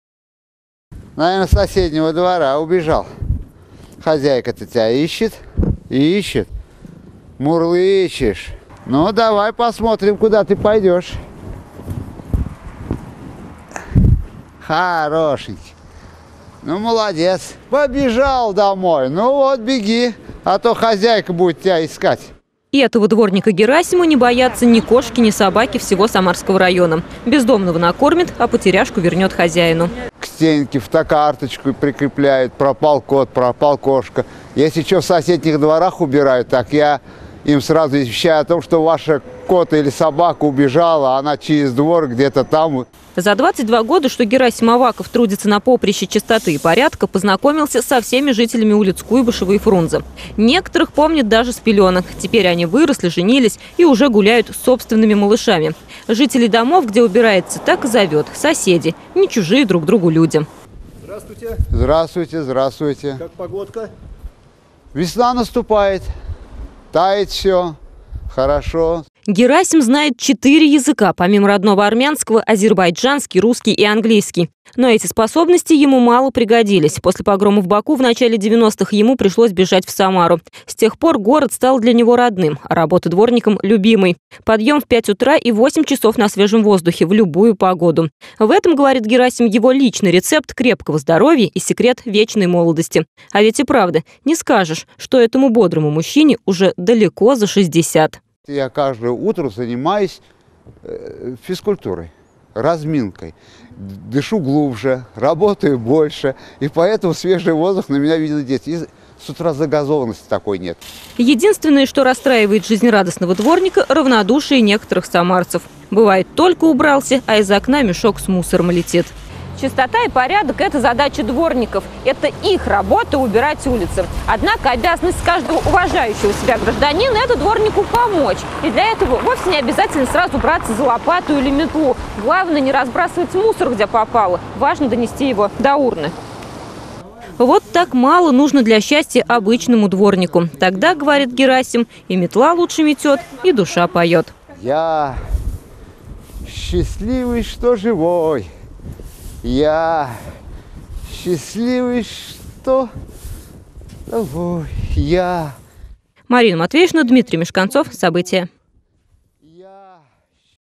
Профессиональные секреты дворника Герасима
Не будешь учиться - в дворники пойдешь. Работа, которой с детства родители пугают своих детей, для Герасима Авакова стала делом жизни. И оказалась не так проста, как можно подумать на первый взгляд